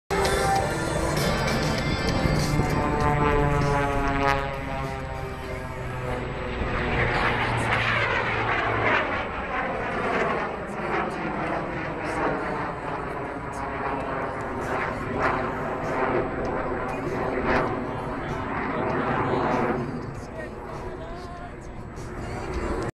The Jack Links Screaming Sasquatch at Fort Worth Alliance Air Show
Has anybody ever seen a jet engine mounted on the tail end of a biplane?